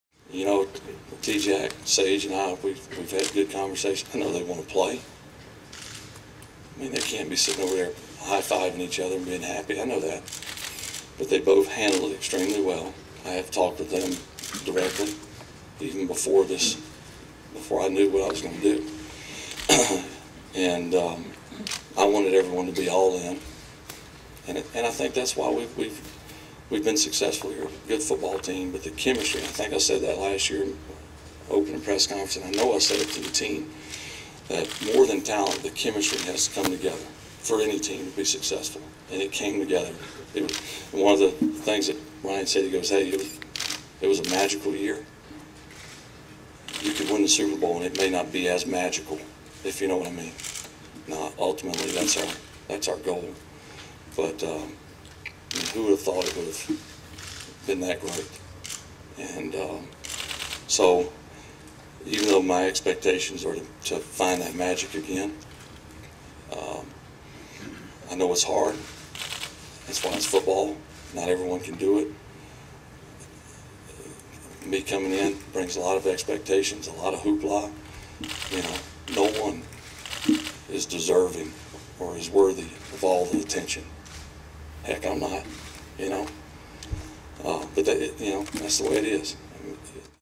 Favre on Jackson and Rosenfels: "They can't be sitting over there high-fiving each other and being h
Favre on Jackson and Rosenfels: "They can't be sitting over there high-fiving each other and being happy. I know that."